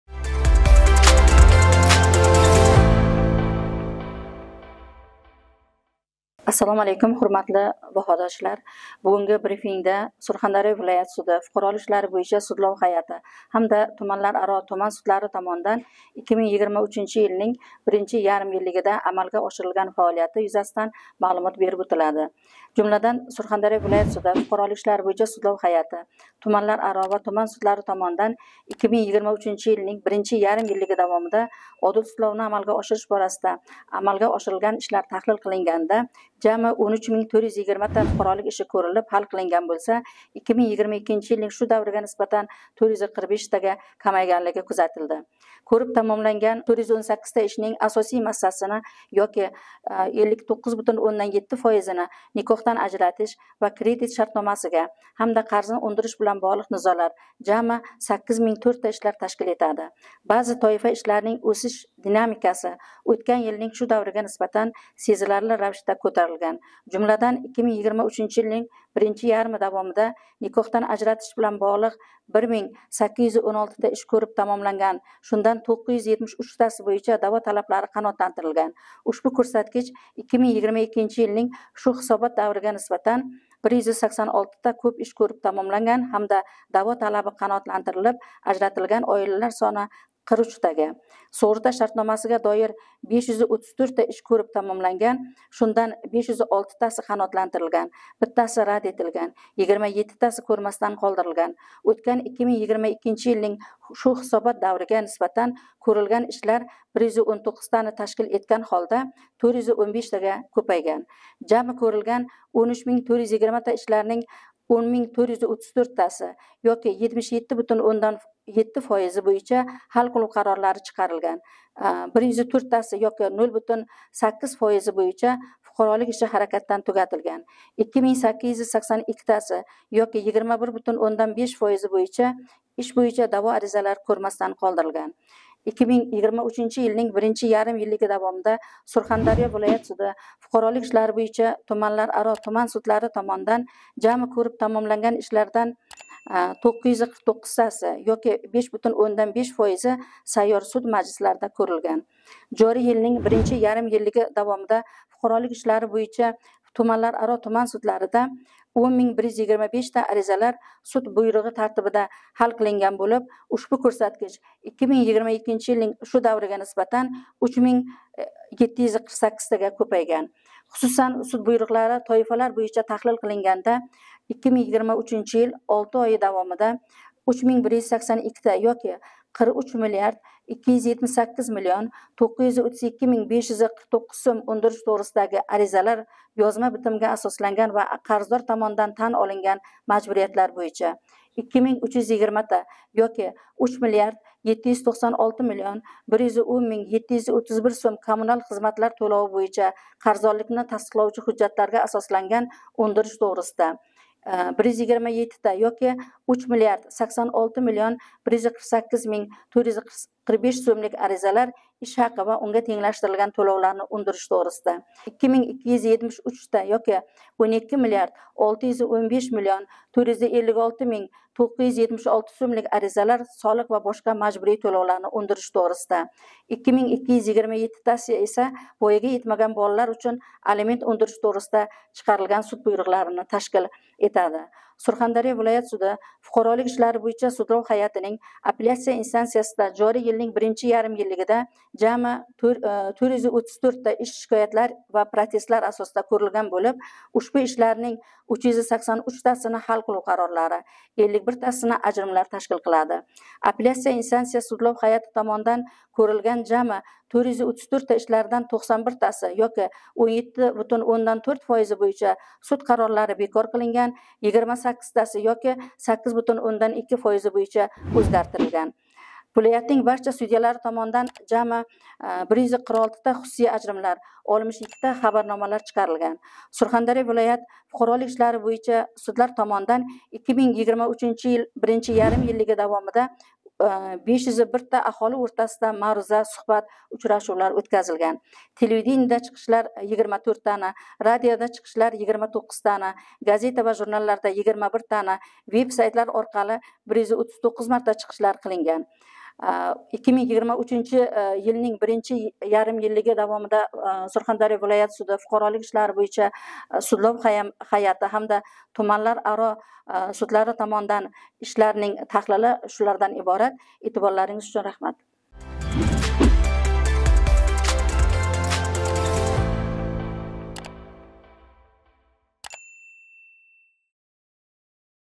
БРИФИНГ
СУРХОНДАРЁ ВИЛОЯТ СУДИ ФУҚАРОЛИК ИШЛАРИ БЎЙИЧА СУДЛАР ТОМОНИДАН 2023 ЙИЛНИНГ I-ЯРИМ ЙИЛЛИГИДА АМАЛГА ОШИРИЛГАН ИШЛАР ТАҲЛИЛИ